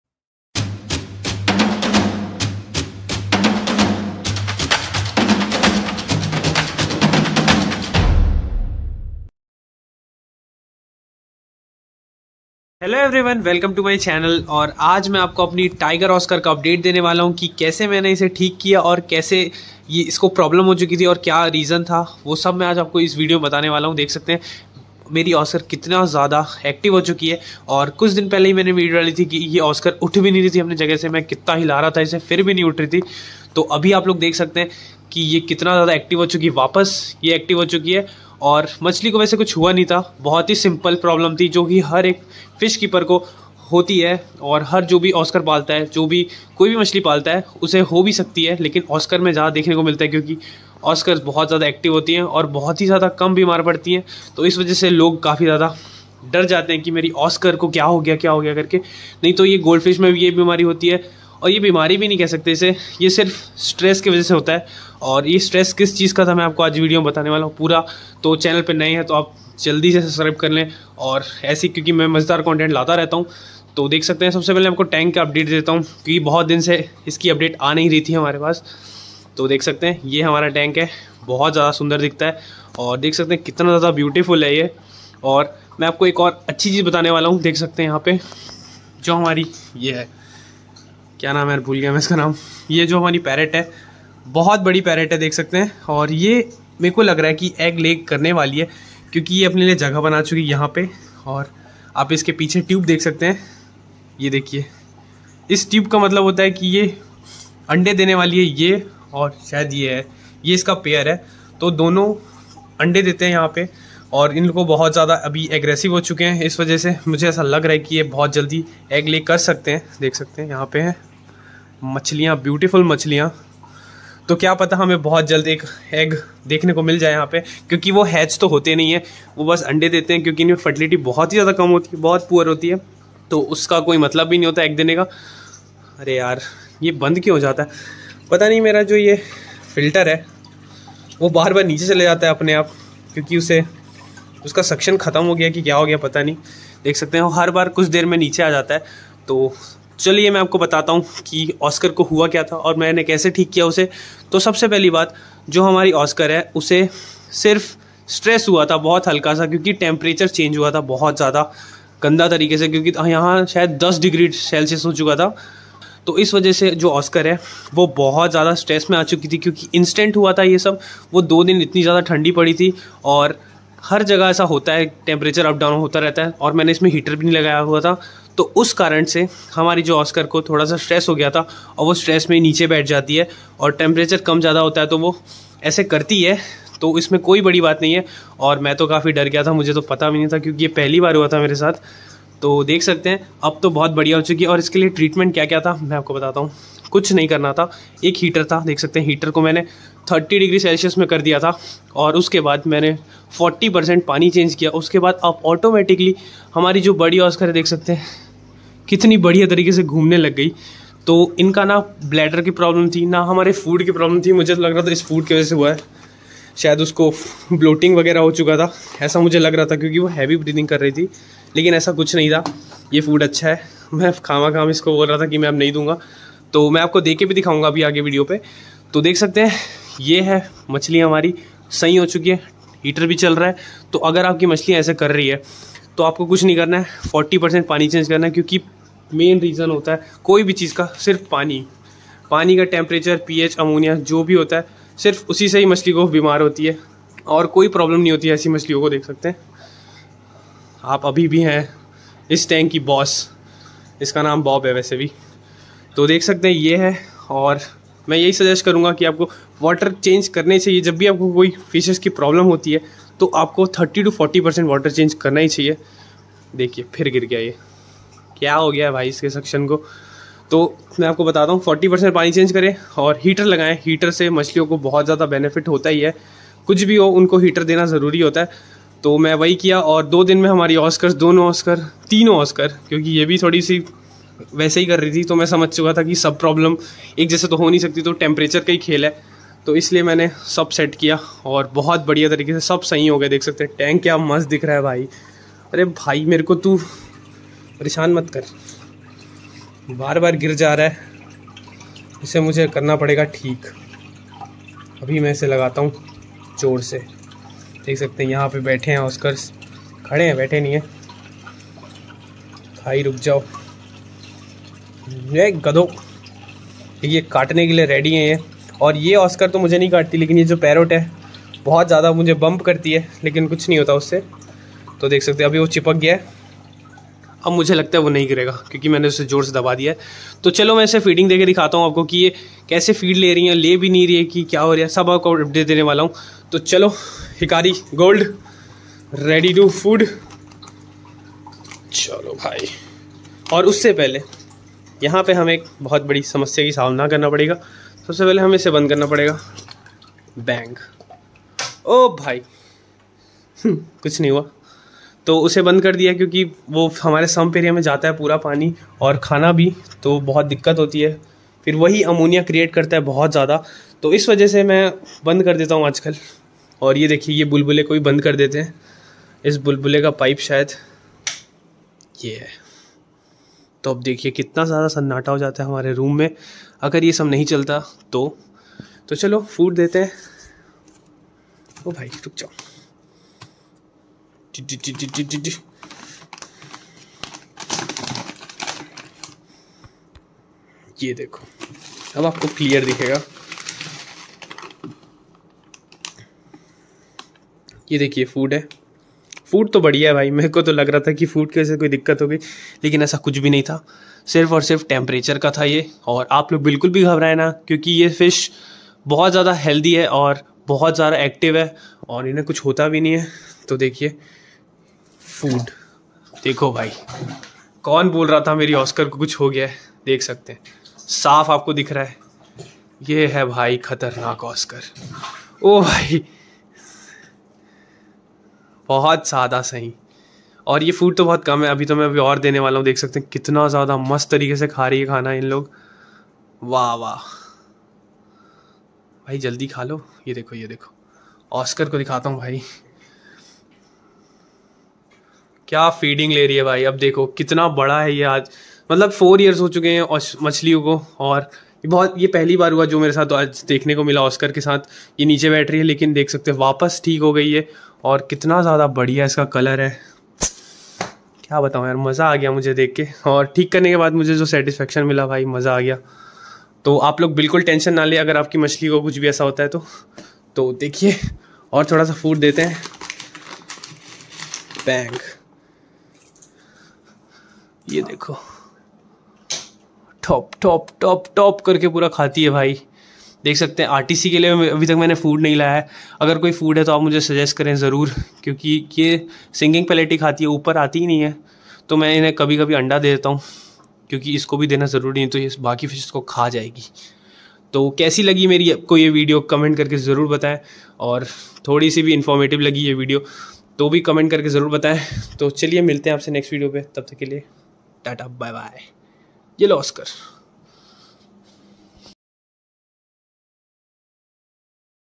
Oscar Fish Lying On the Bottom 🥺|Easy Treatment & Care |Oscar Thik Hogyi😍|Nikhil Patle
Oscar Fish Care and Treatment in Winters Season 💥🙏Easy 🔥